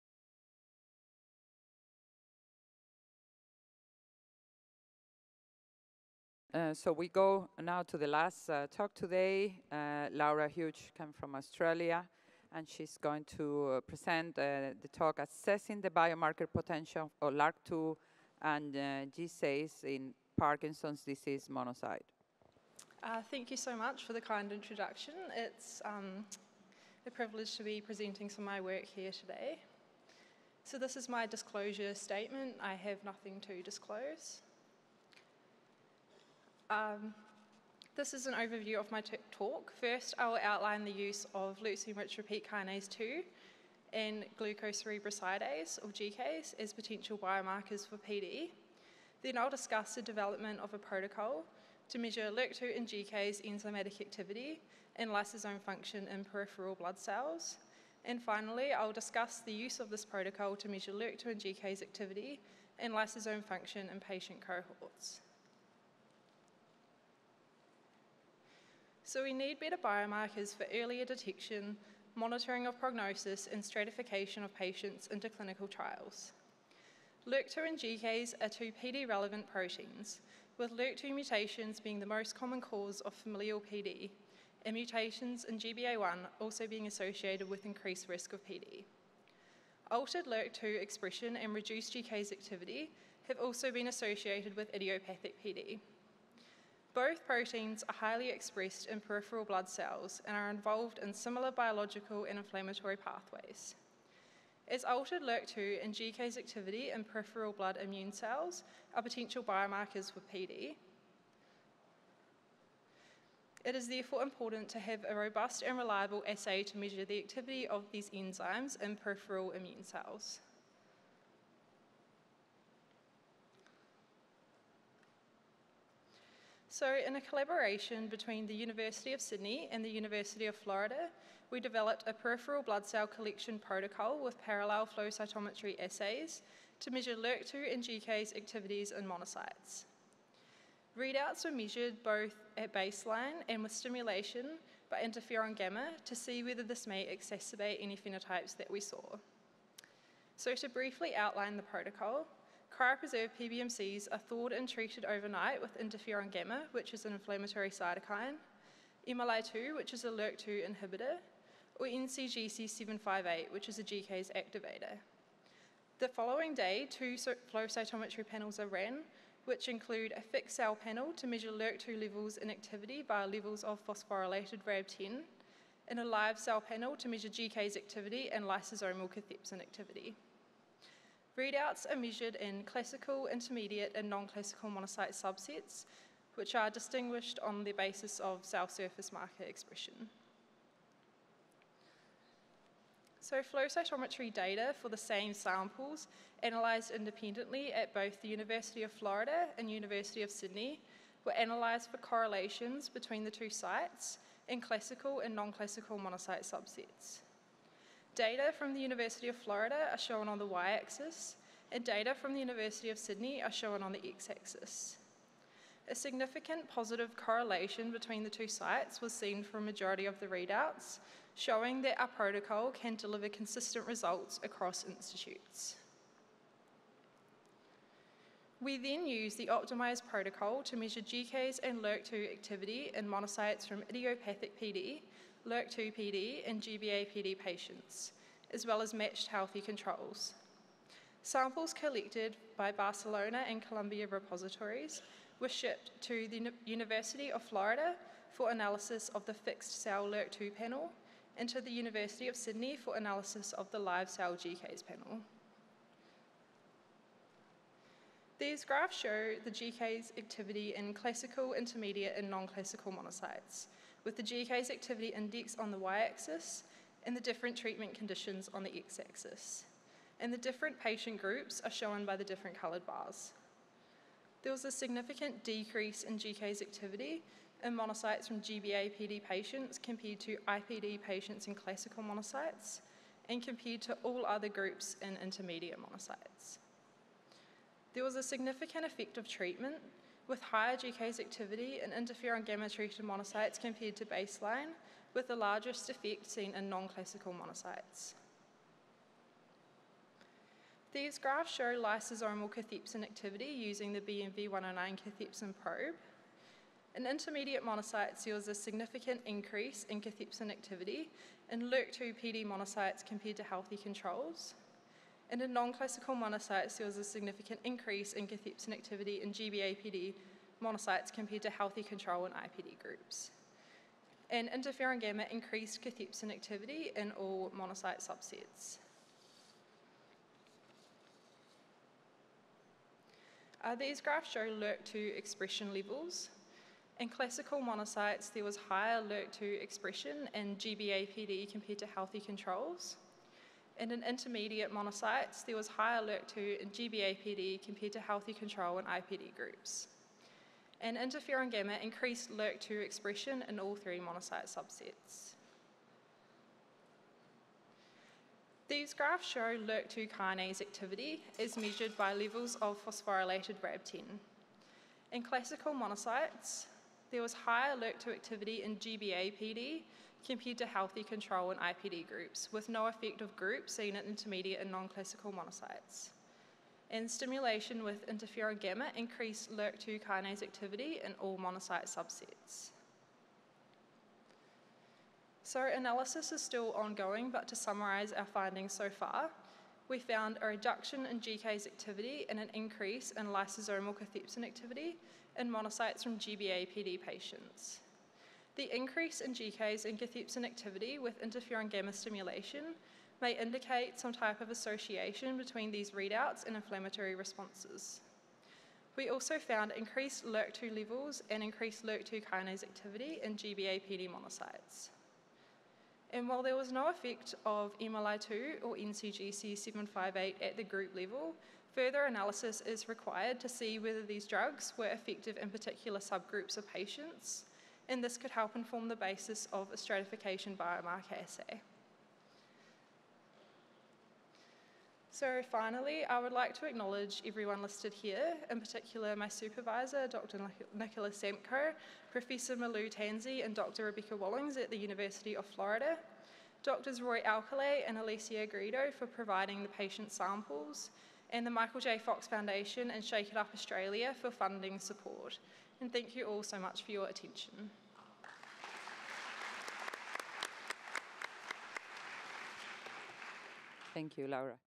WPC 2023 Hot Topics - Laura Hughes
Assessing the biomarker potential of LRRK2 and GCase in Parkinson's disease monocytes